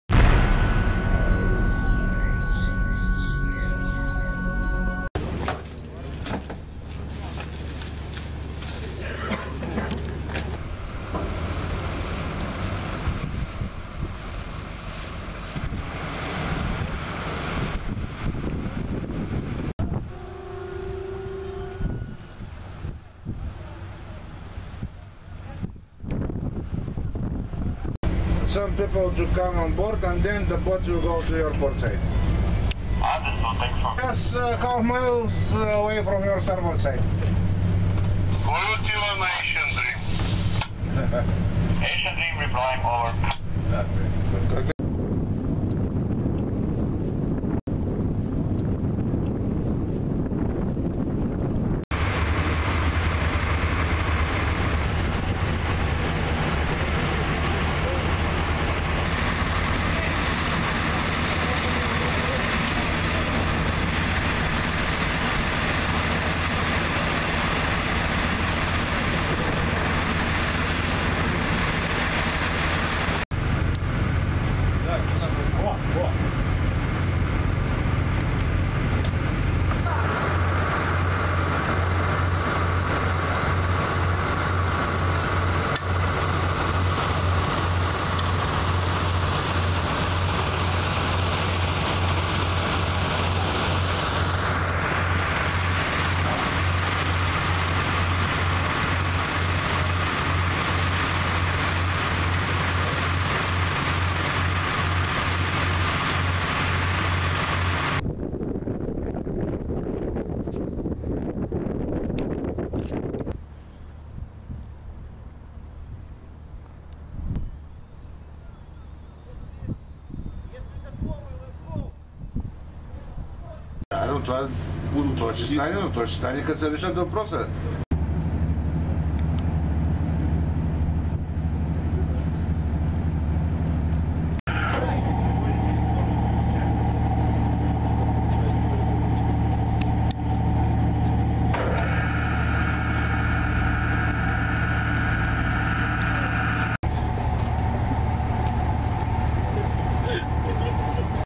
Кораб ASIAN DREAM Варна
Днес бяха осигурени провизии на украинските моряци от кораб ASIAN DREAM, които бяха оставени от собственика и мениджъра на плавателния съд. Усилия в спасяване на 14-те украинци обединиха „Строителен и технически флот” АД, Български червен кръст и Морска администрация.